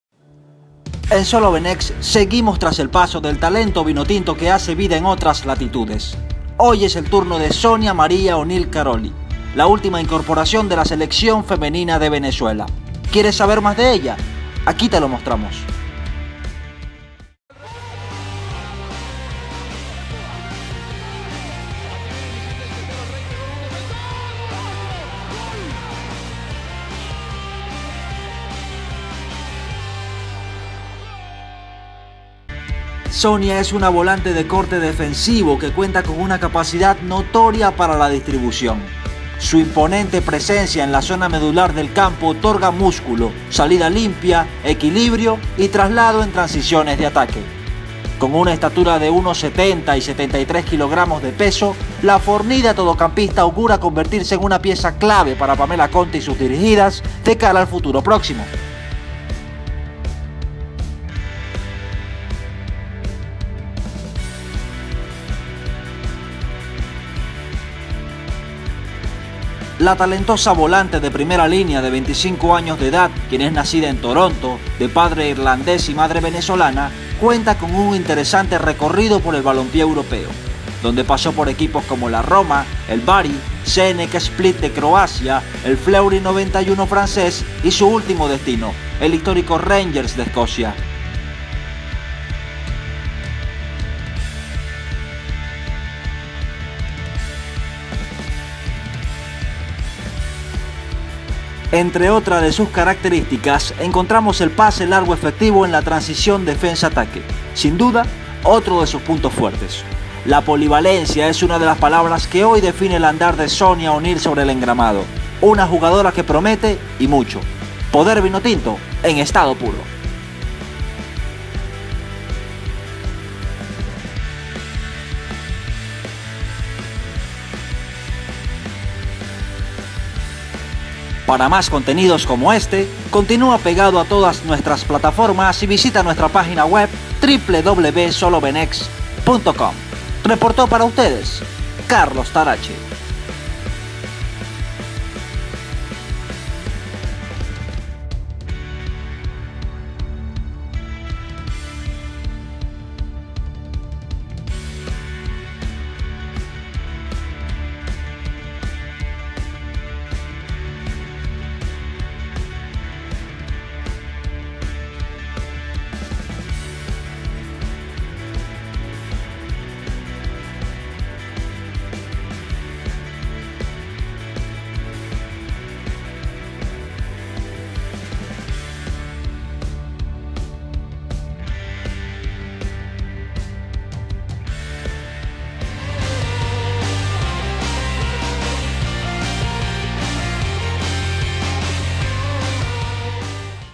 Conoce a Sonia O'Neill, la flamante jugadora de la selección venezolana | SOLOVENEX
Nuestro director Carlos Tarache analiza el juego y la trayectoria de nuestra guerrera Sonia O'neill. Así como todo lo que puede aportar en la escuadra dirigida por Pamela Conti.

🎙Carlos Tarache ( @ctaracher )

🖥️ Luis Salas ( @iamzurdo_20 )